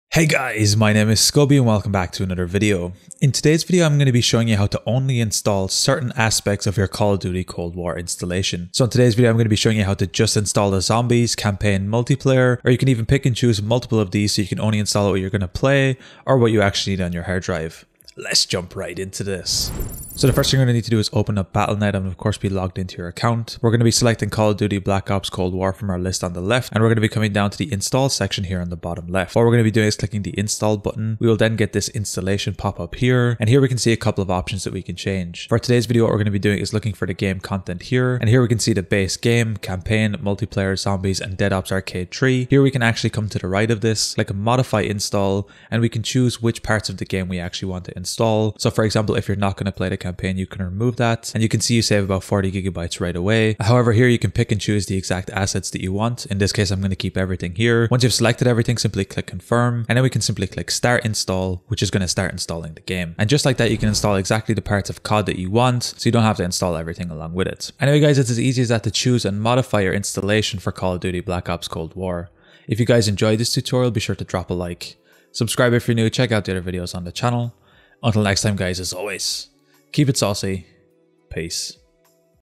Install Certain Parts of CoD Black Ops Cold War(Only Install Multiplayer, Zombies etc.) PC!
Hey, guys, my name is Scoby Tech and in today's video, I am going to be showing you how only install certain parts of the CoD black ops cold war. This method will allow you to only install multiplayer, zombies, or campaign and pick and choose which parts to install. This can help save on download time and disc space on your PC!

0:00 Intro
0:23 Battle.net Install Settings
1:03 Outro